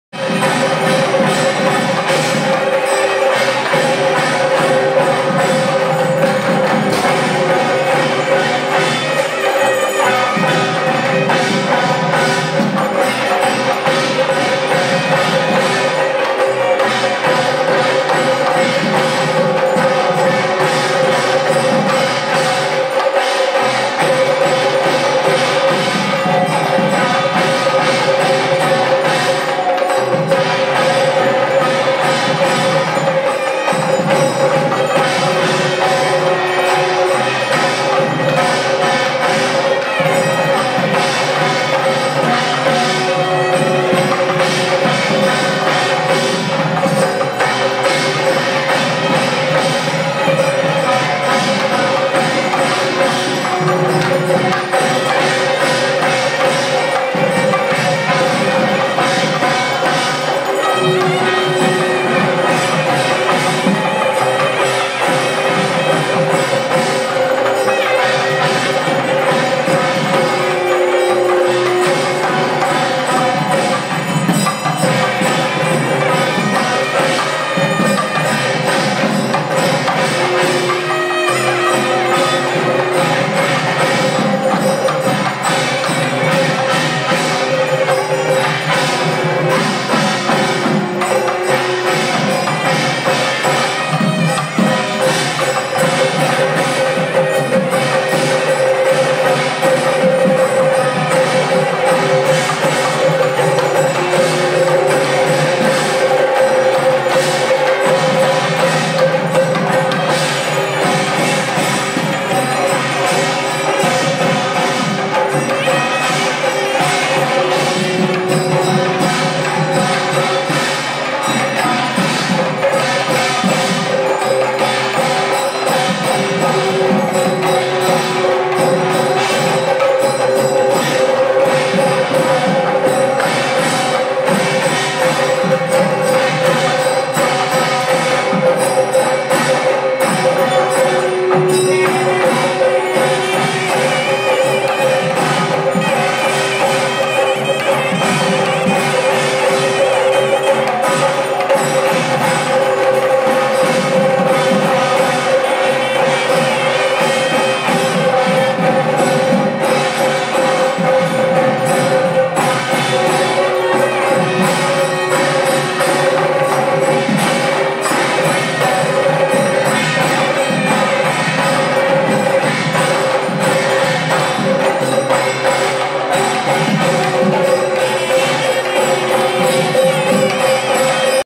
Sri Raghavendra Swamy Aaradhan Celebration atSri Panduranga Ashrama Pillaiar Koil Street Chennai
Sri Raghavendra Swamy Aaradhan and Sri Krishna Janmashtami Celebration at
Sri Panduranga Ashrama
(Unit of Sri Pejavara Matha, Udupi) New No. 100, Pillayar Koil Street, Vadapalani, Chennai - 600 026.